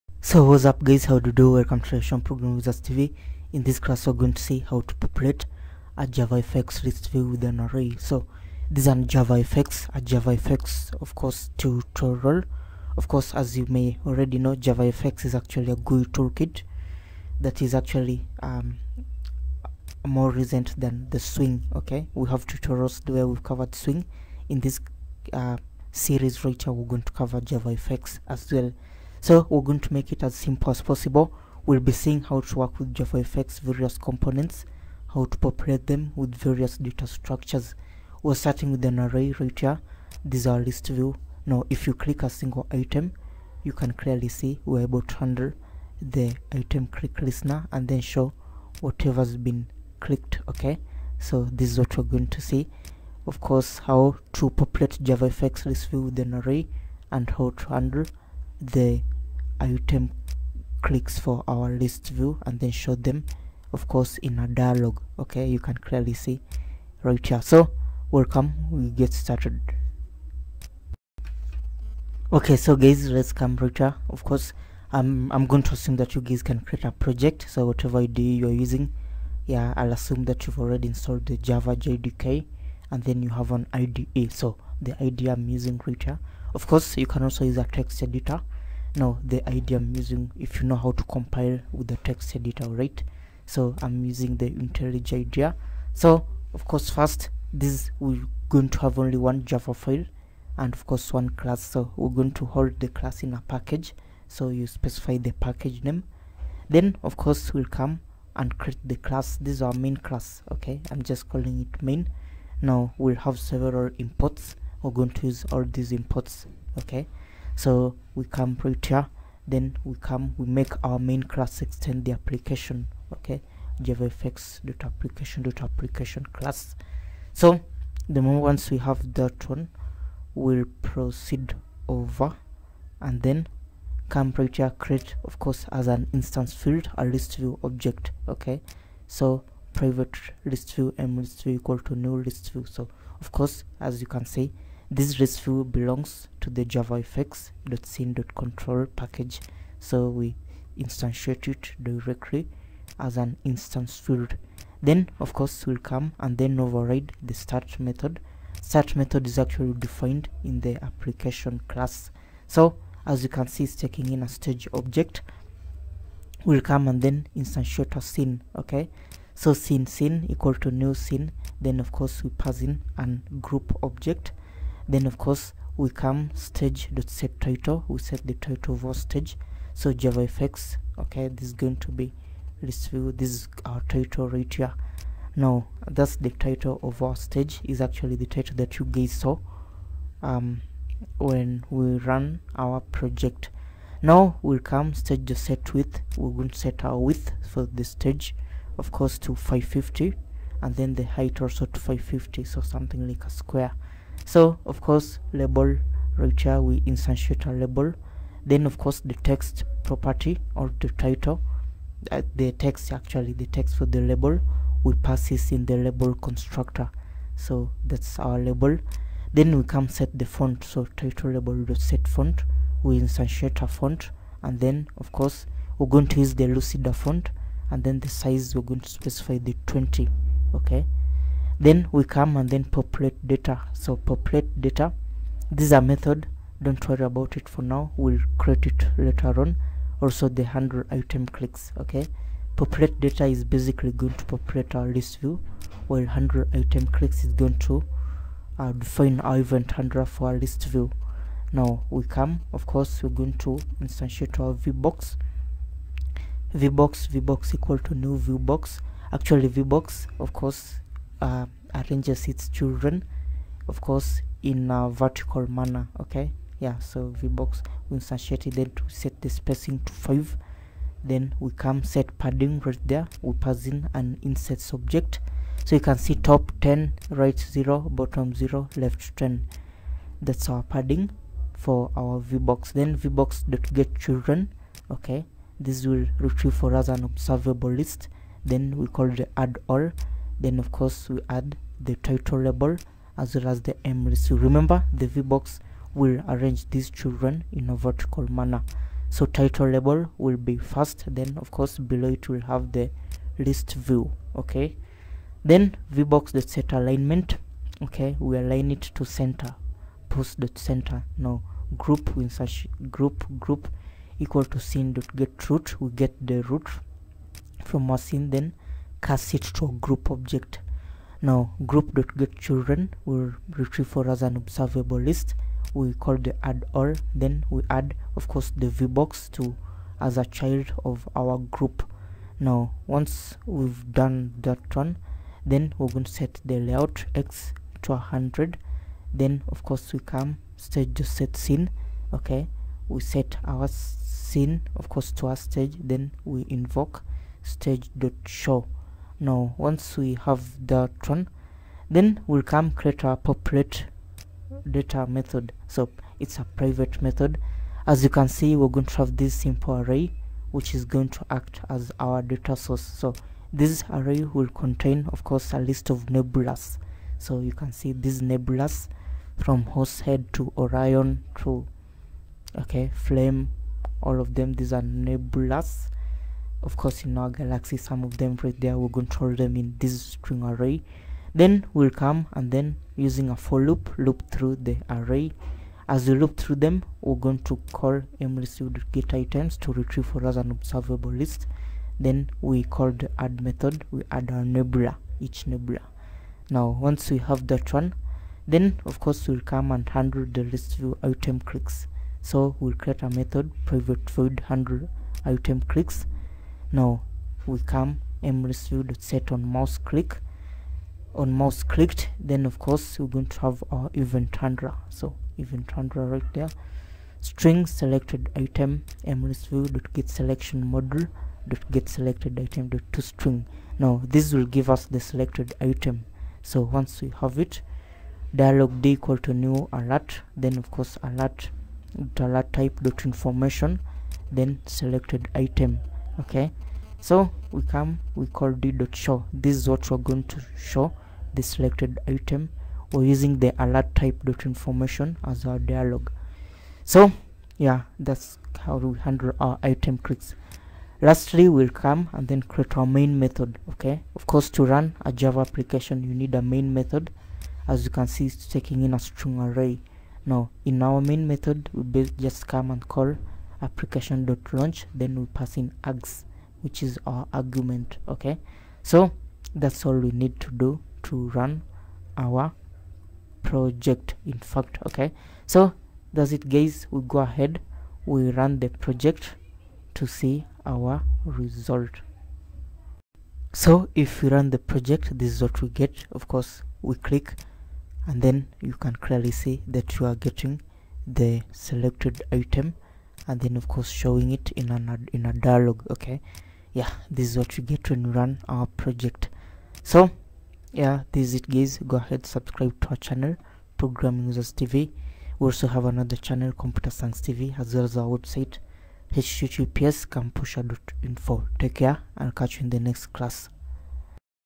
JavaFX : ListView - Fill From Array ,Handle ItemClicks
So welcome guys. This is a JavaFX tutorial. JavaFX is a modern Java GUI toolkit which will ultimately replace Swing. It's much more powerful and modern and also  easier to program in.

JavaFX is now the number one GUI toolkit for creating rich graphical java applications.

It allows us create graphical user interfaces programmatically using predefined classes in an object oriented manner.

Sun MicroSystems had acquired JavaFX back in the year 2008.  The company was however incorporated into Oracle in the year 2010 and newer and newer versions of JavaFX are continually being released.

Oracle had open sourced JavaFX in the year 2013 and in fact JavaFX is now part of the JDK so all you need to create JavaFX applications is JDK.

You can use editors like Visual Studio Code or full blown IDEs like Intellij IDEA to create JavaFX applications.

This is an example of how easy it is to use. In this example we look at how to populate a JavaFX  ListView from an array. We handle the ListView itemclicks thus showing a JavaFX dialog with the selected items.

Cheers.